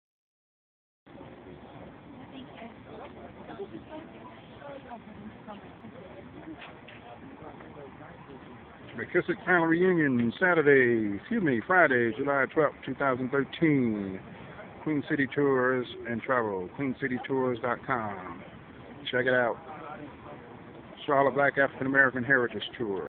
Queen City Tours and Travel McKissic Reunion 2013 CBHT 5 0 Fri 07 12 2013 AM PM 3
Members of the McKissic Family Reunion 2013 enjoying their QCT Charlotte Black/African-American Heritage Tour on Fri. 07/12/2013.